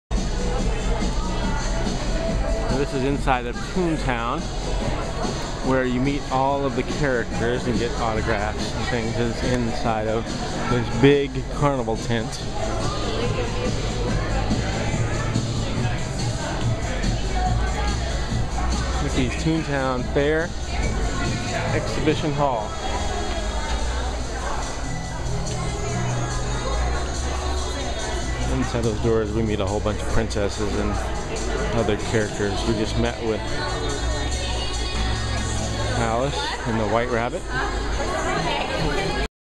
43 - The Exhibition Hall
Inside of Toontown is the Hall of Fame Tent and Exhibition Hall where kids can meet more characters and get pictures and autographs.